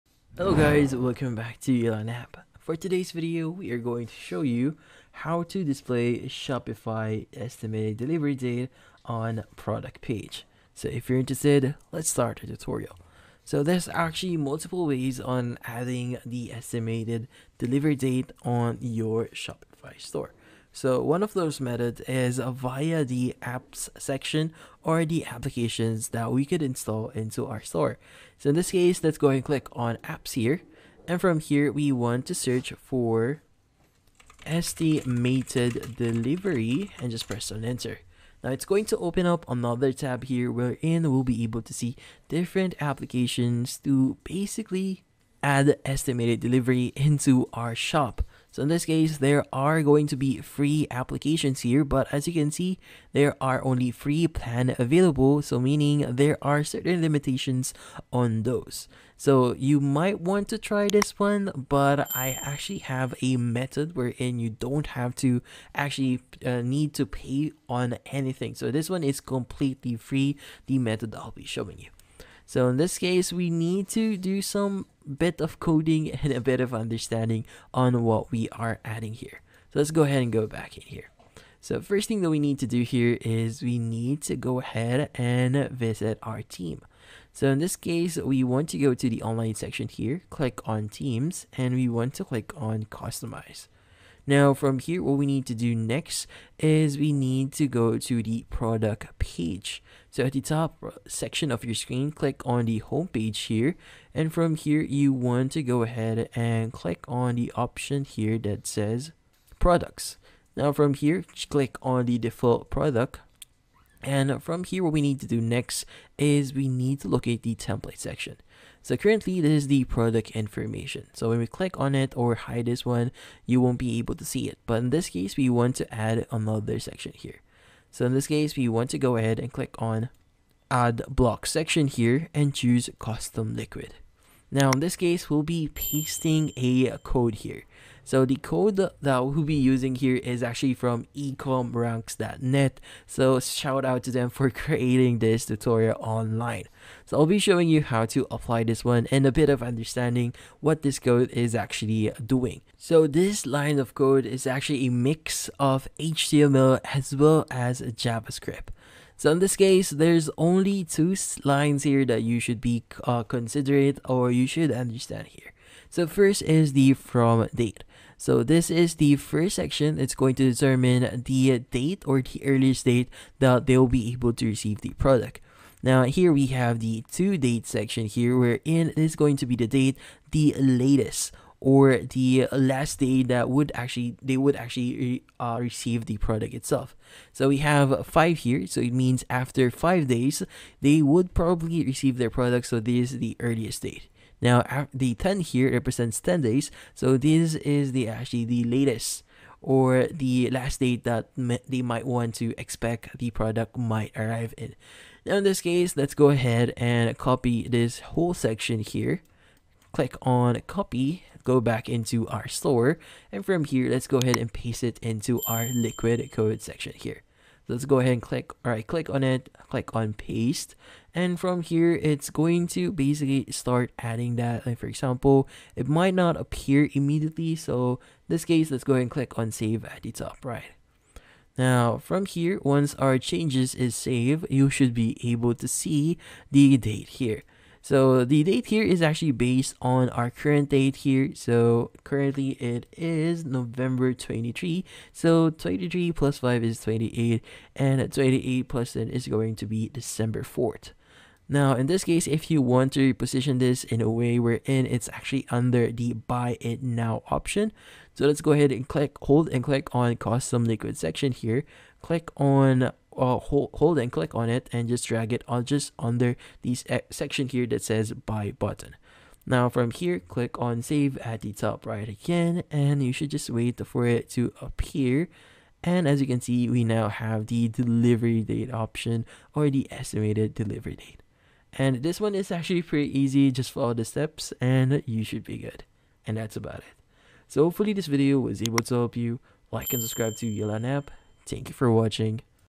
How To Display Shopify Estimated Delivery Date On Product Page (2025)
If you want to be able to inform your customers about the estimated delivery date directly on the product page of your Shopify store, then this video will be perfect for you!